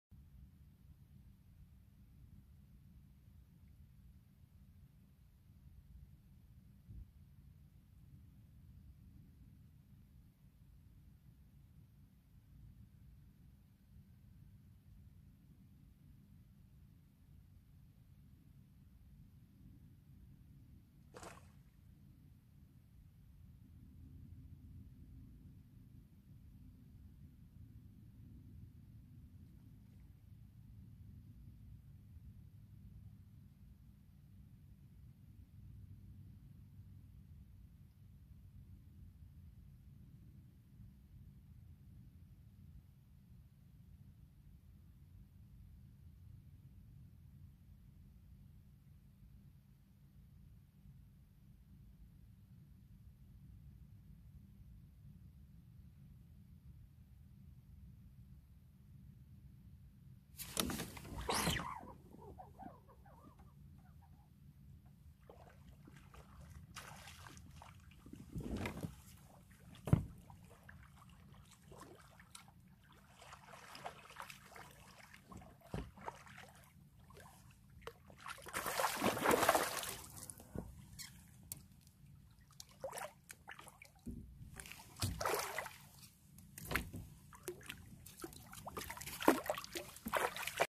※부림낚시터 야간 장대낚시※ 장찌를 천천히 올려주는...
왠지 부림낚시터를 가면 누군가를 만났을 것 같았는데...
제 예감이 맞았네요.
대물 잉어(닉네임)님을 뵙고 여러 이야기도 나누고 좋은 시간이였습니다.
그리고 예전에도 느꼈지만 부림낚시터 사장님 참 편안하고 친절하시네요...